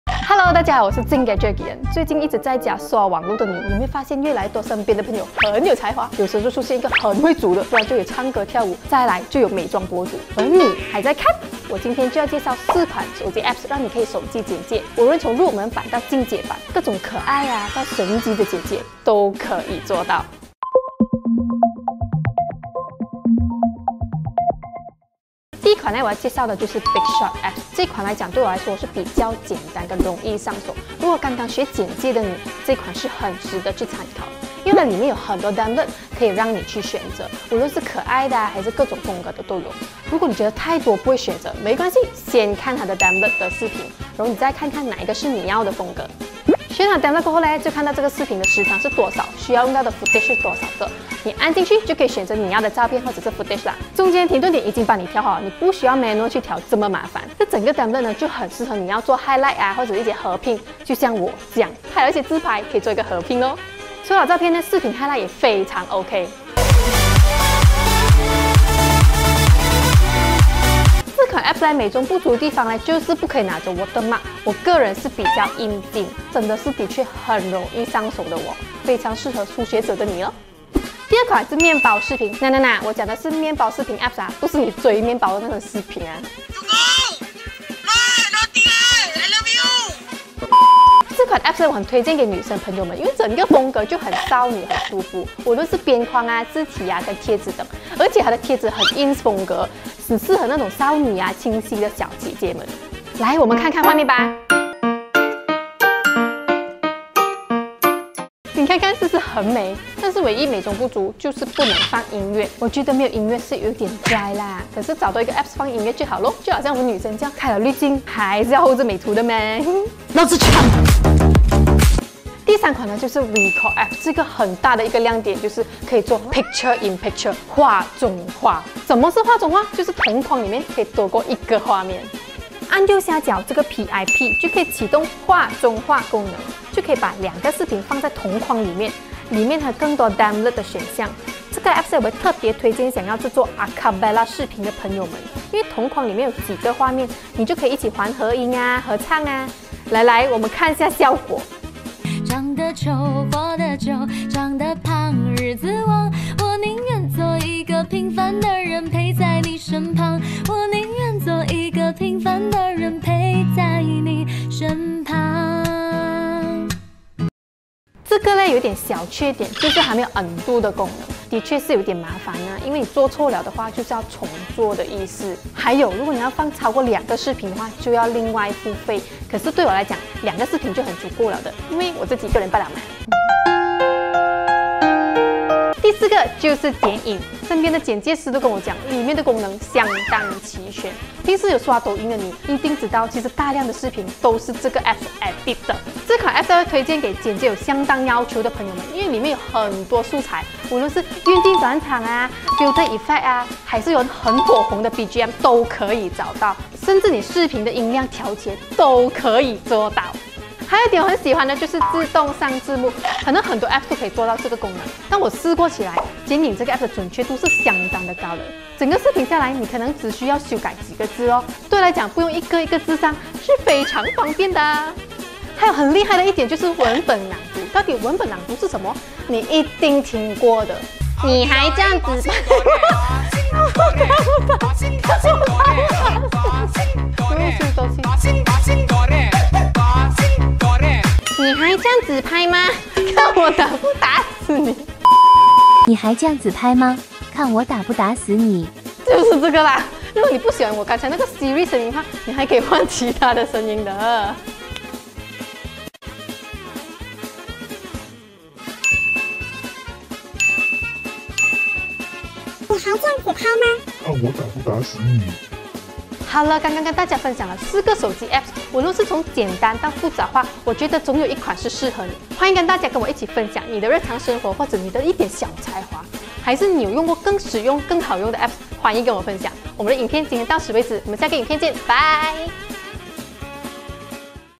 超方便！的四款手机剪辑APP
这段期间发现身边很多很有油菜花🥦的朋友！
小小分享关于手机剪辑的APP! 让你在家制作视频～ 让自己的生活变得更有趣❤️

欢迎大家分享你的生活👻 或者更多有趣的APP ☺️

在视频分享的APP有以下：
📏Bigshot
📏 Wecol
📏面包视频
📏剪映

我们会继续给您带来更多科技相关作品！！